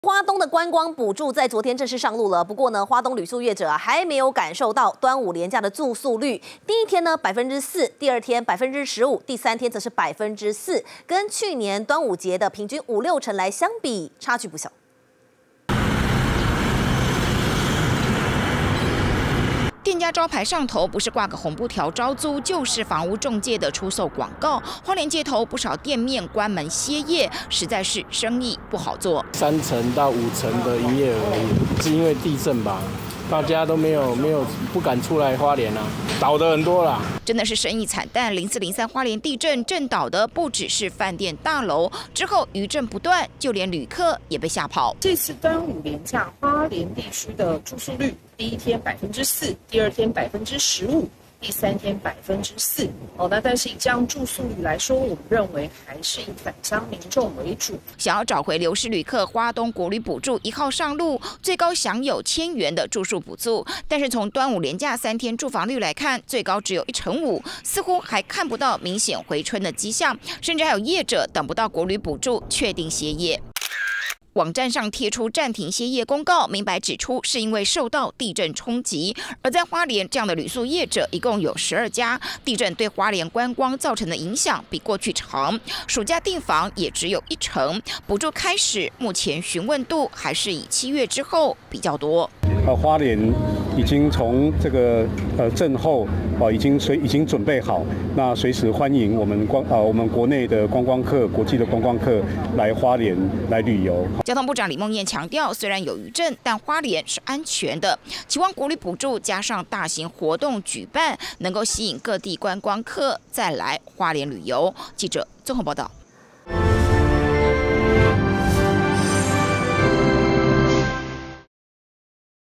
地震陰霾！ 端午連假住宿率「僅1.5成」 花蓮12家旅宿歇業
花東觀光補助昨（1）日正式上路，不過，花蓮旅宿業者似乎看不到明顯回春的跡象。今年端午連假的住宿率，第一天4％，第二天15％，第三天4％，跟去年端午節住房平均5到6成，差距不小。受到地震影響，旅客沒有回流，目前有12家花蓮旅宿業者，等不到國旅補助確定歇業，衝擊不小。

下載【年代電視】APP：
iOS平台：